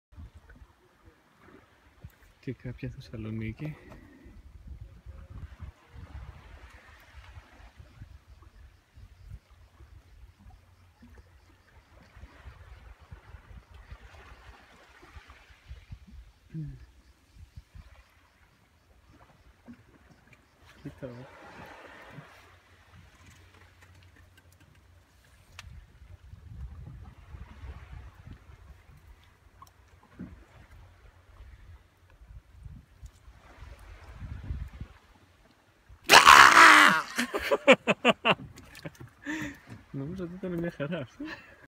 Crystal clear sea at Agios Sostis, Serifos.
Watch the crystal clear waters of Agios Sostis beach, a sandy and peaceful beach of Serifos island.